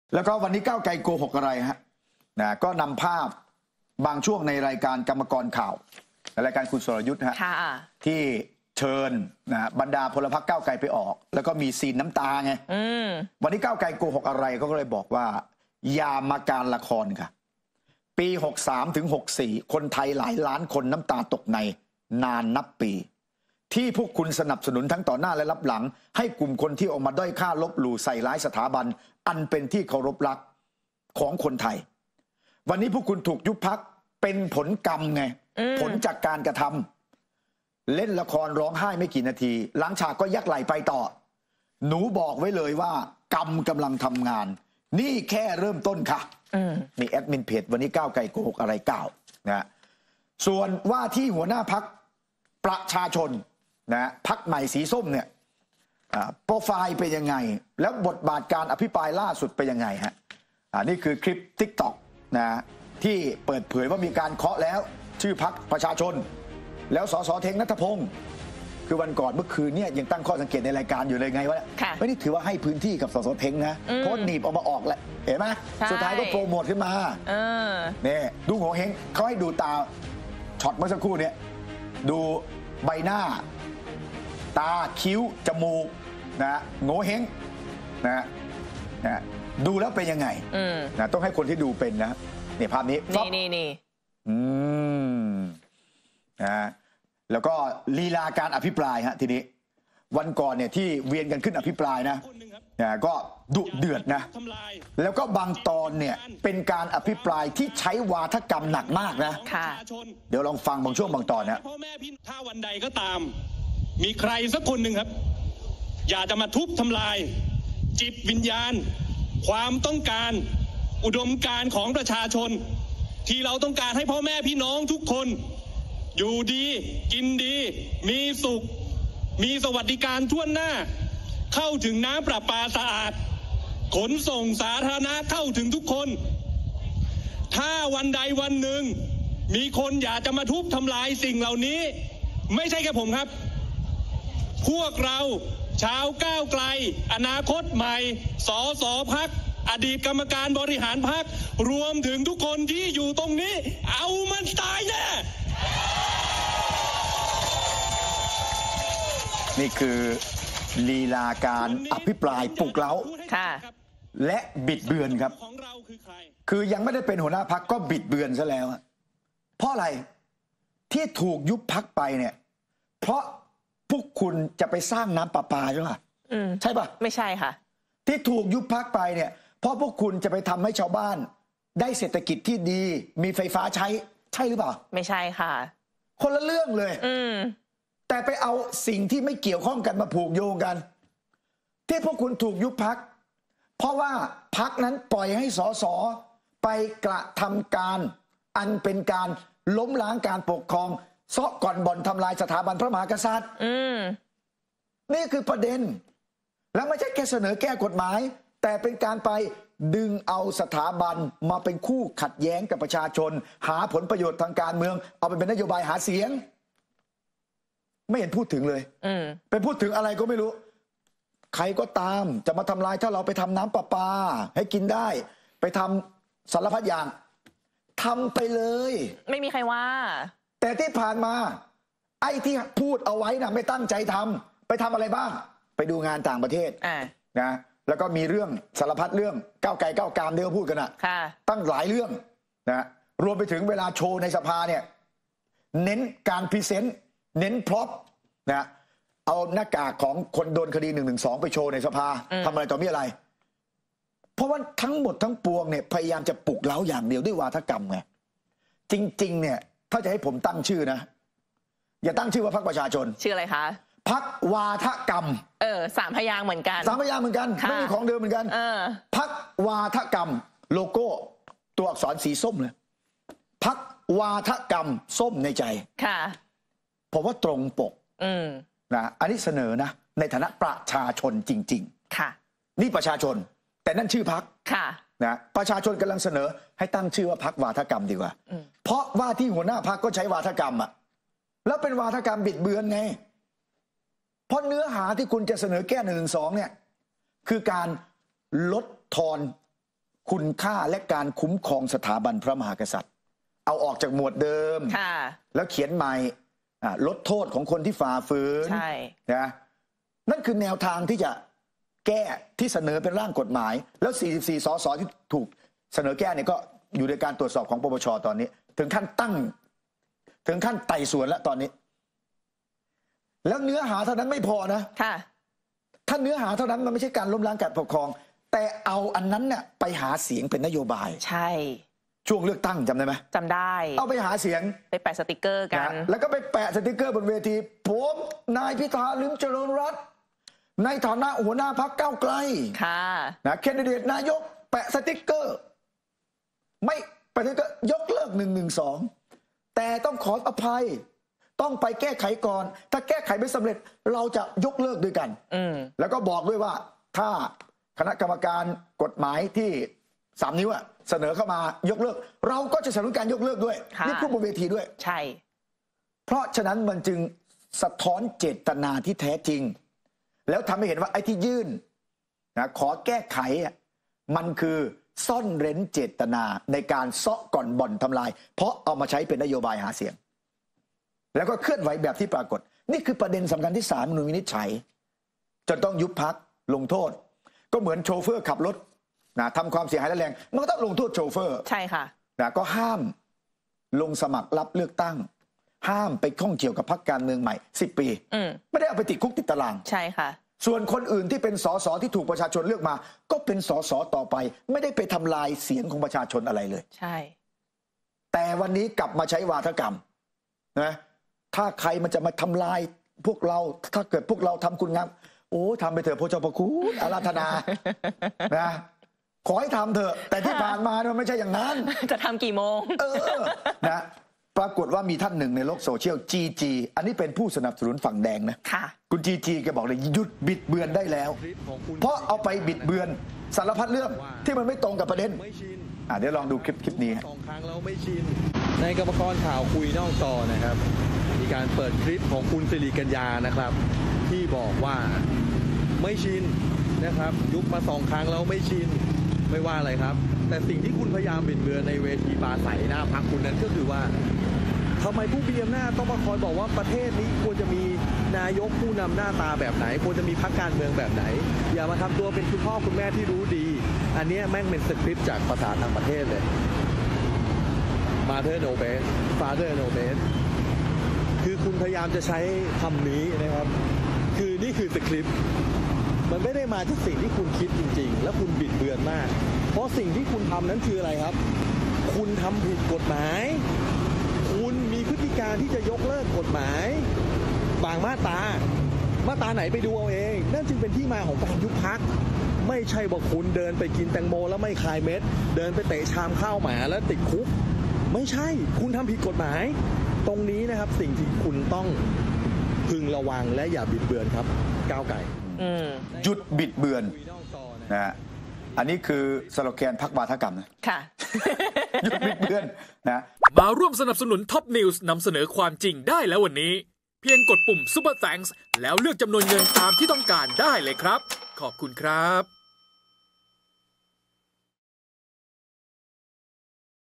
"สันติสุข" อัดยับ "สส.เท้ง" หัวหน้าพรรคประชาชน(ส้ม)แนะชื่อใหม่พรรควาทกรรม ตรงปกที่สุด
แค่เริ่มก็บิดเบือนแล้ว "สันติสุข" อัดยับ "สส.เท้ง" หัวหน้าพรรคประชาชน (ส้ม) แนะชื่อใหม่พรรควาทกรรม ตรงปกที่สุด